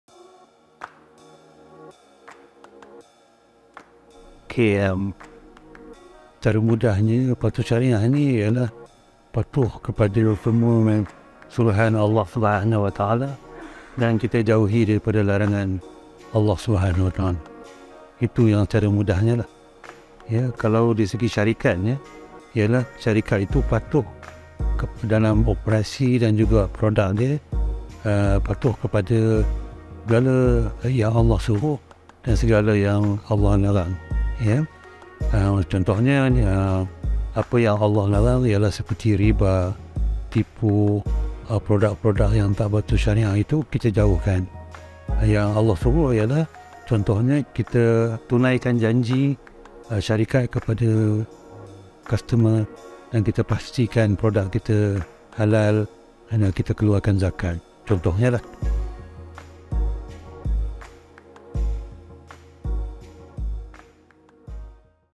What is Shariah Compliance in Business?
Let's hear what Ustaz Lariba said about it.